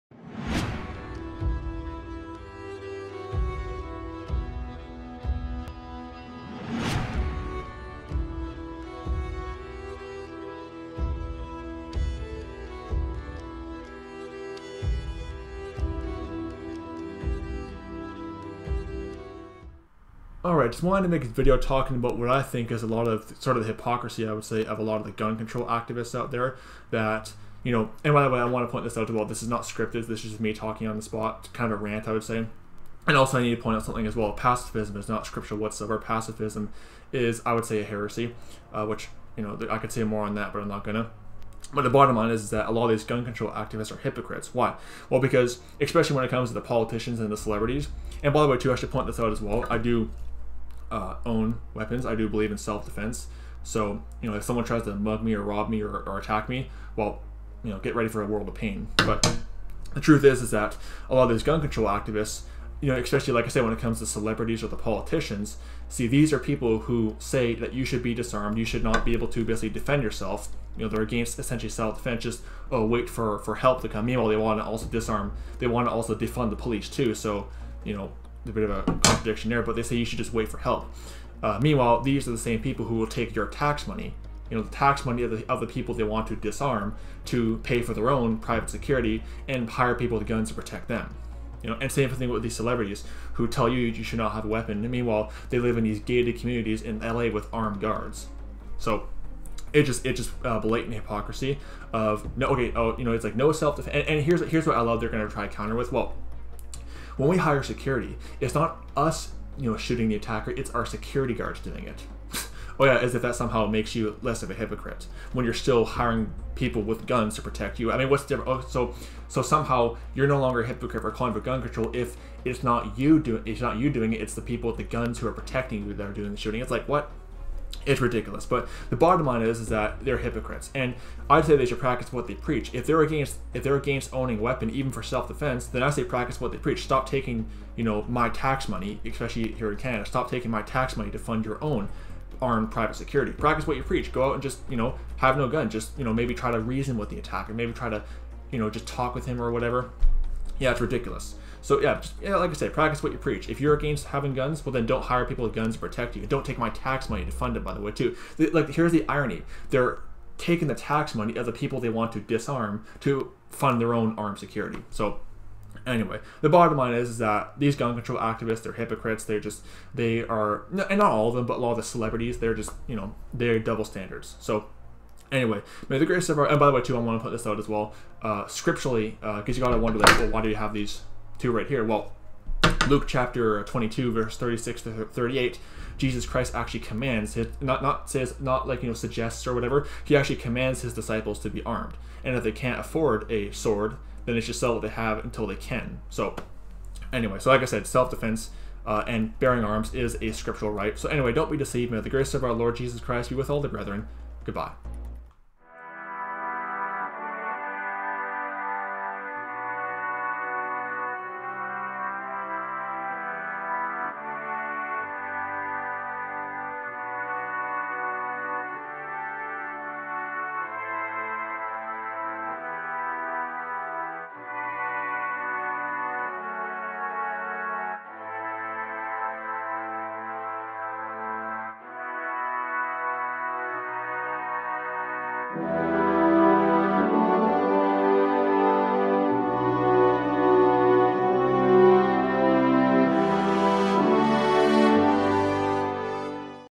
The Hypocrisy Of Many Gun Control Supporters
The hypocrisy of many of the ultra-rich HELLywood celebs (Ariana Grande, Madonna, Rihanna, and others) when it comes to gun control and usage of guns for self-defense purposes.